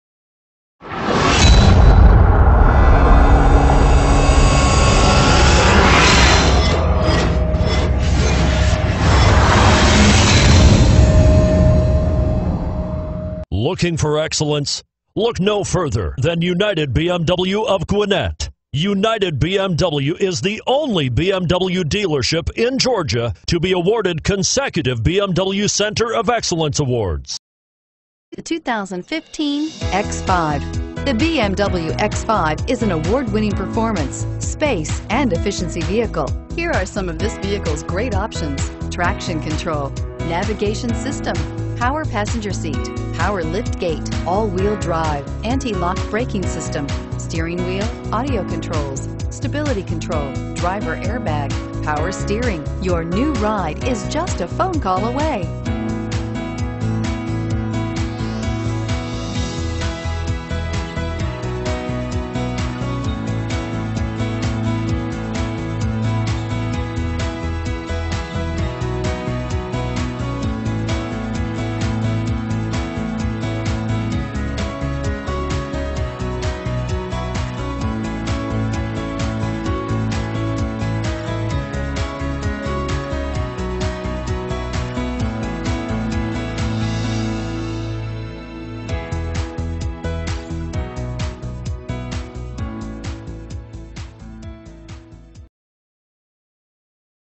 2015 BMW X5 Duluth GA 13162
2015 BMW X5

As the most aggressive New Car dealer in the Southeast our prices won't be beat! This 2015 BMW X5 xDrive35i Sport Activity Vehicle has a Dark Graphite Metallic exterior and Black interior!